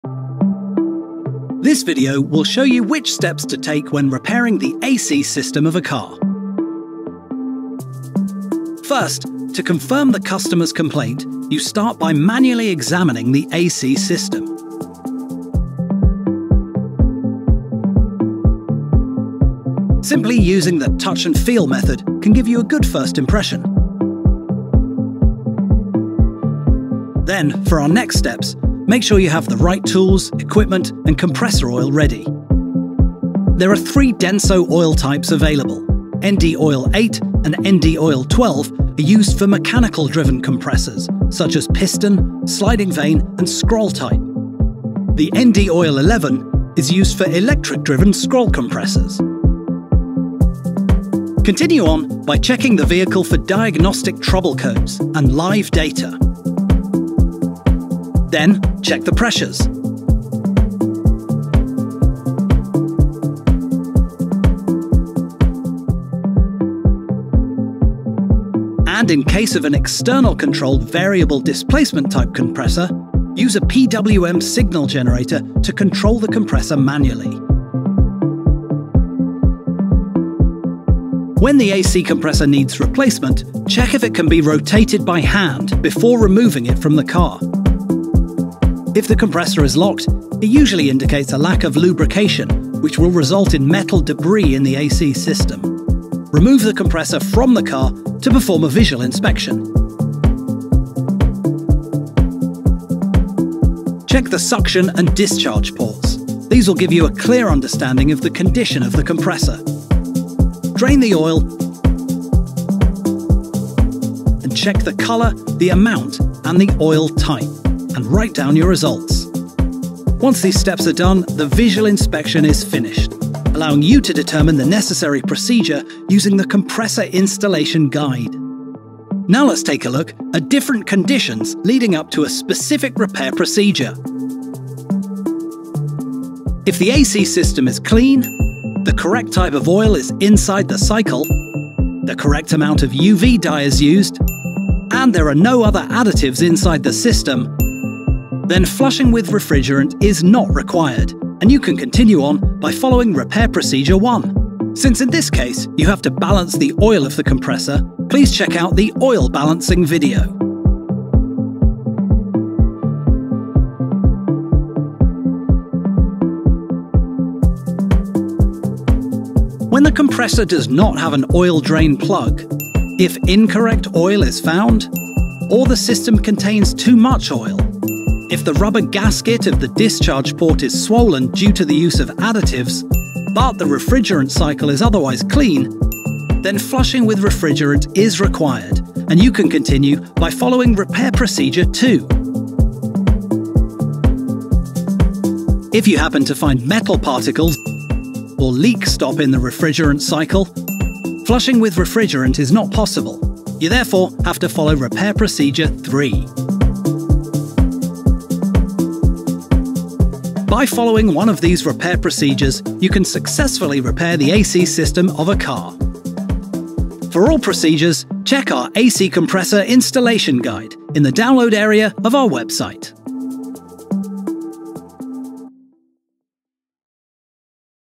DENSO Aftermarket - how to replace an AC compressor
This video shows you step-by-step how to replace an AC compressor. It provides valuable insights and tips to ensure a successful compressor replacement. Trust DENSO Aftermarket to equip you with the knowledge and confidence needed to tackle this task with ease.